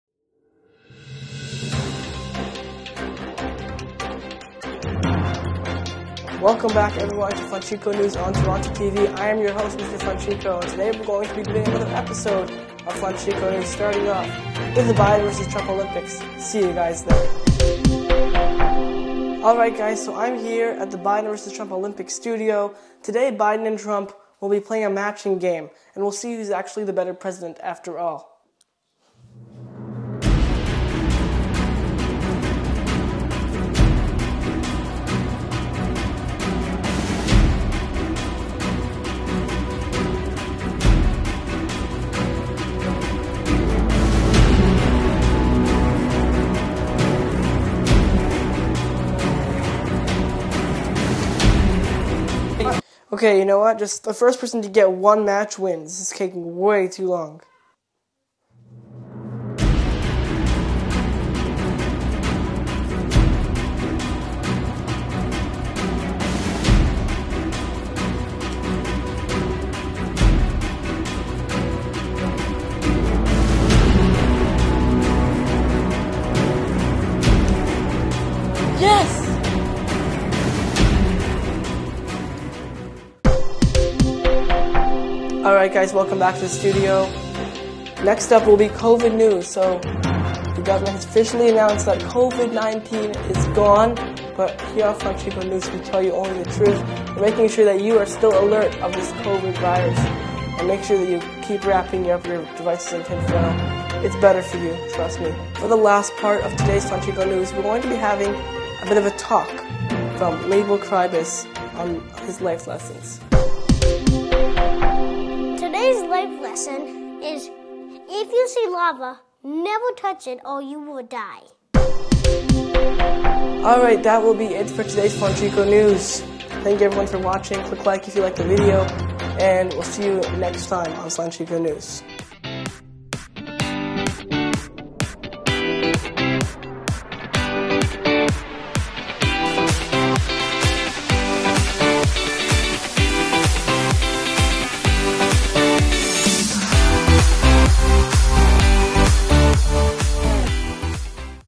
Surprise! | Flancheeko News | Episode 11
We're back. for now! Hope you guys enjoyed the newest and shortest edition of Flancheeko News. Plus a new segment with "Leibel's Life Lessons."